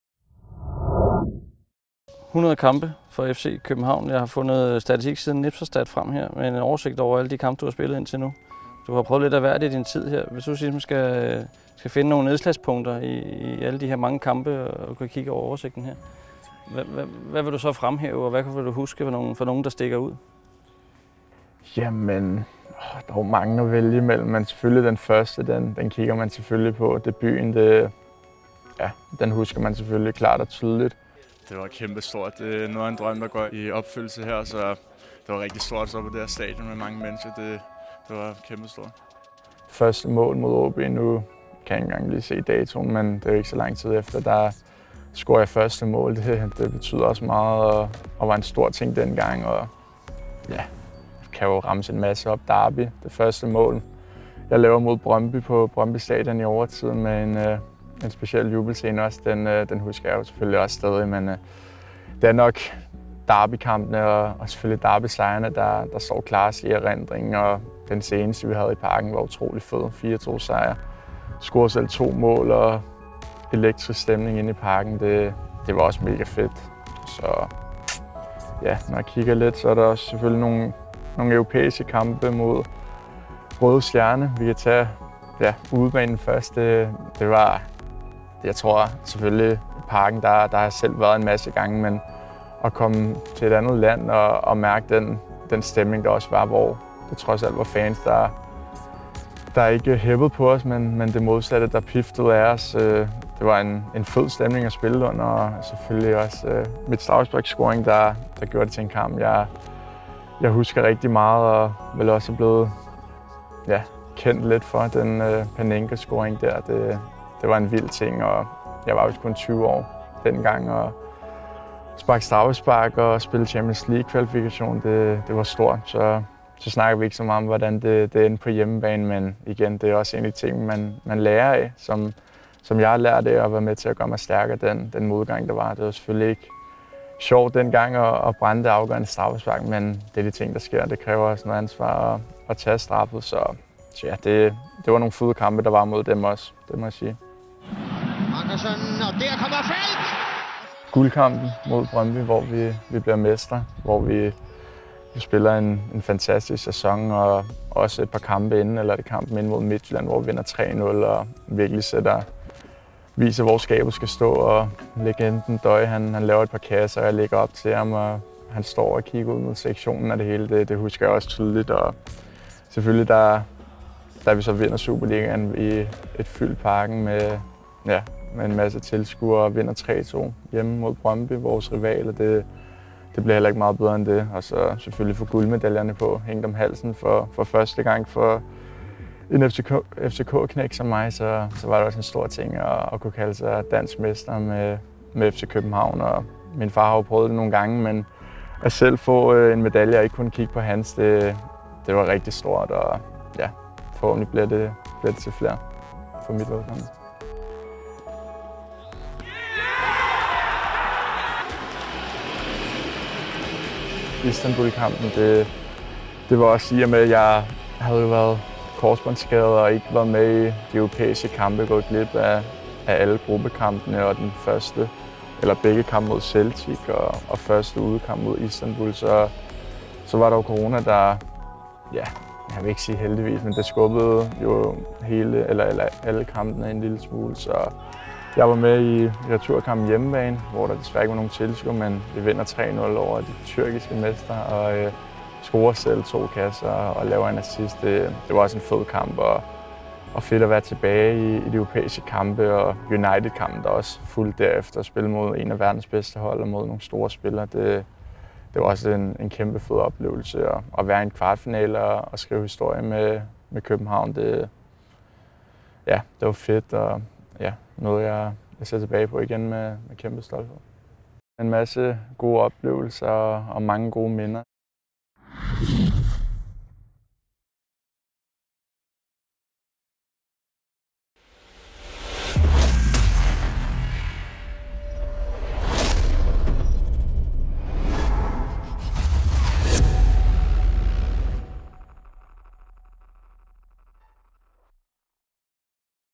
Jonas Winds 100 kampe: De største oplevelser
I anden del af det store interview med jubilaren Jonas Wind ser han tilbage på de største oplevelser fra de første 100 kampe for klubben. Hvad er dit bedste Joni-minde?

profilnavn: fc_kobenhavn